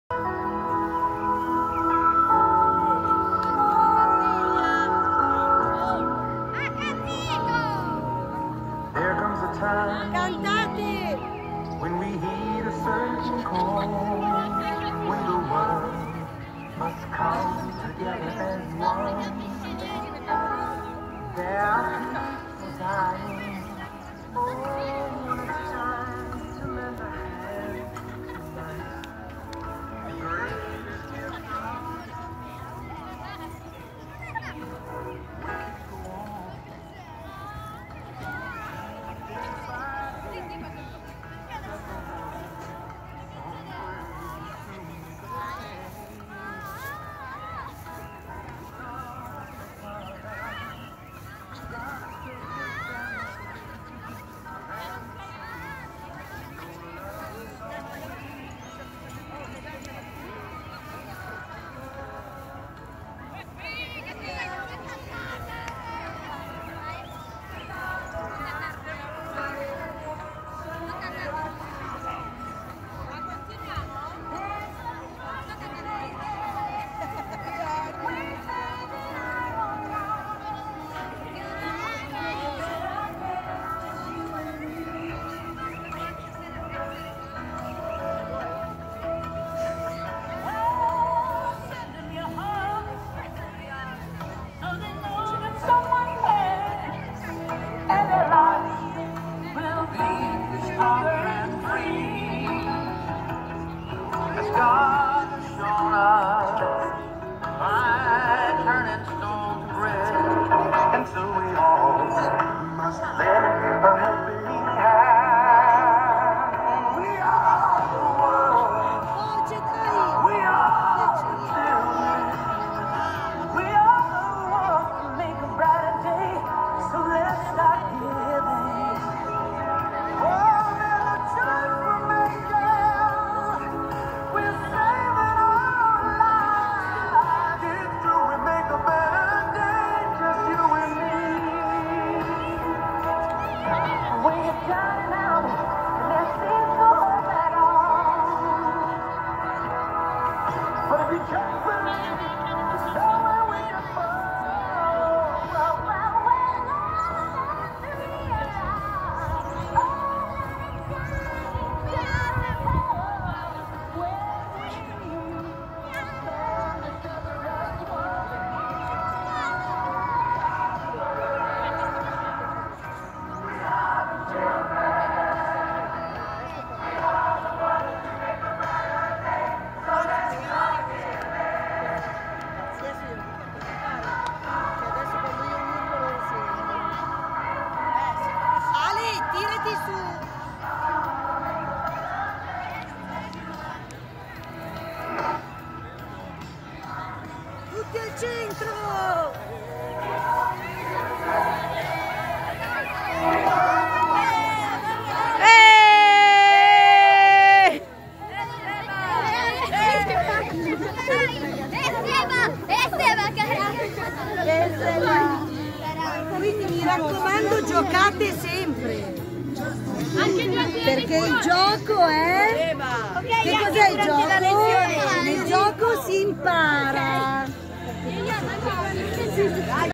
La giornata del diritto del gioco 27/05/22
Giornata indetta dal comune di Milano per le scuole dei 9 municipi. I nostri alunni consiglieri del Municipio 7 non potevano mancare!